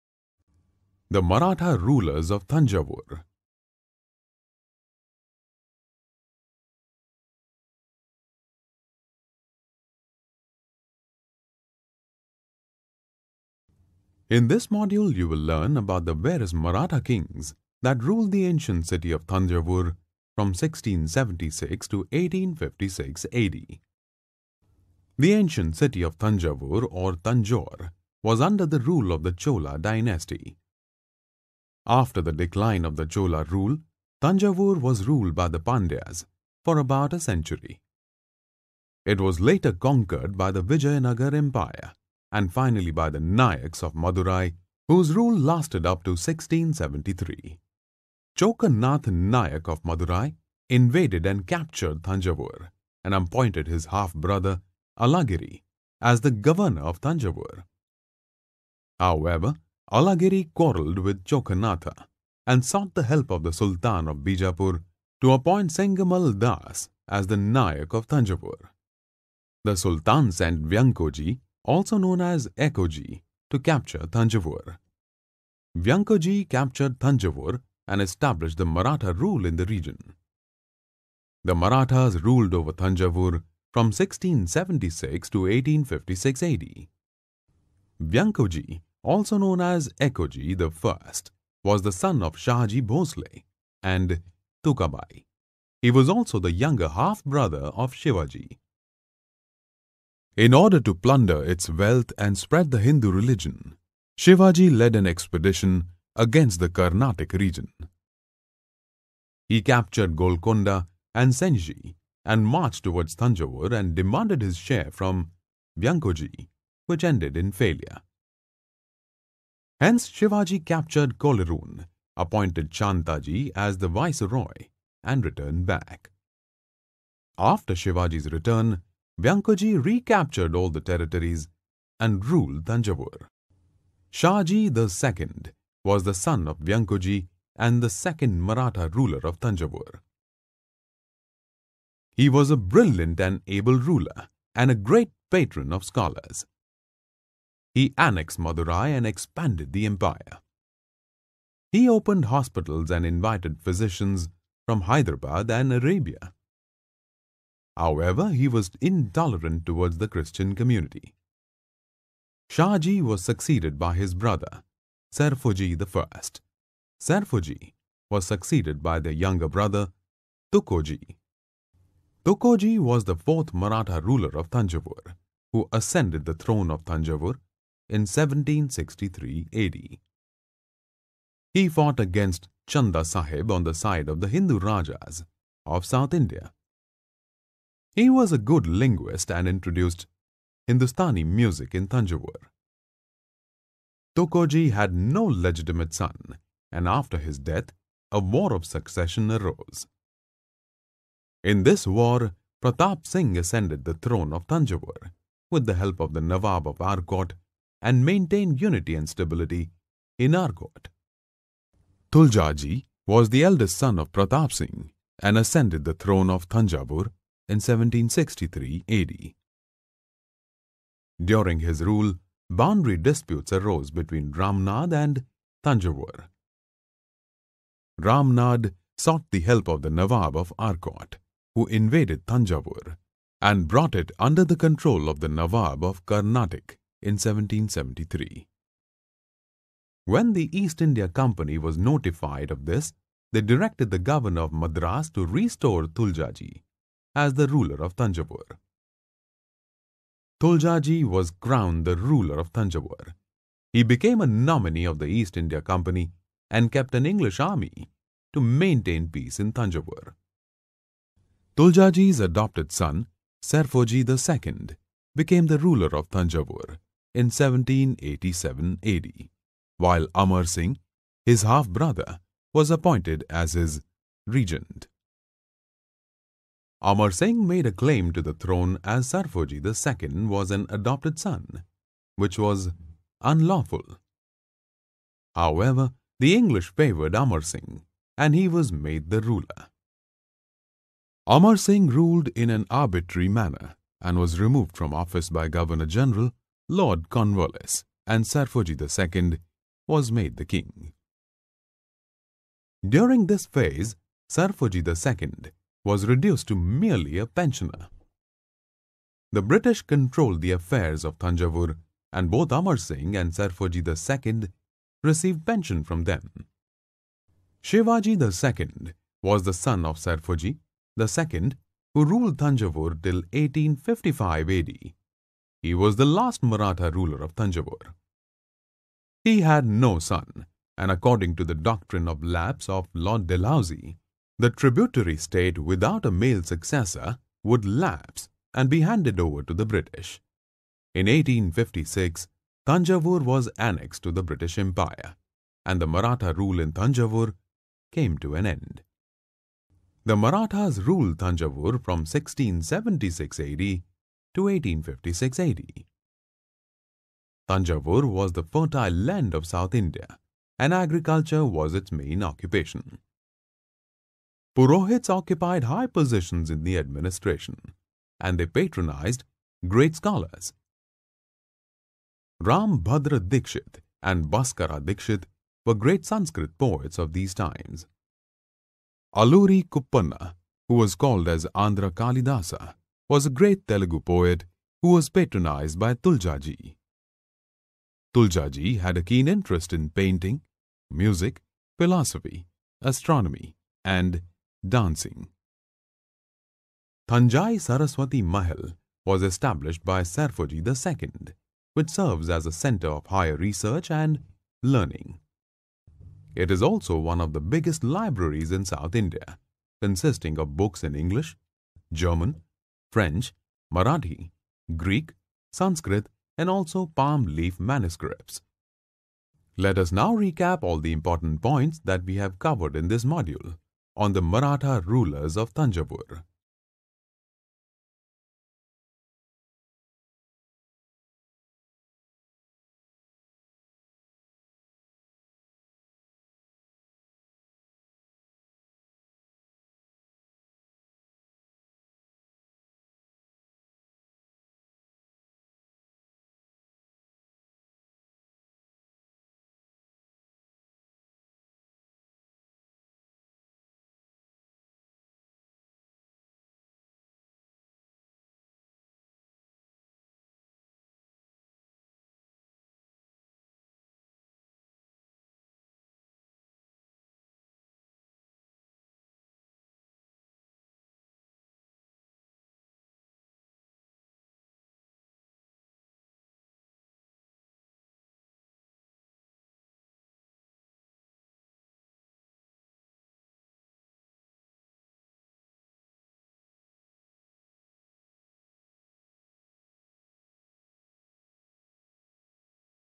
The Maratha Rulers of Thanjavur
This interactive animation explains about the various Maratha kings that ruled Thanjavur from 1676 to 1856 AD.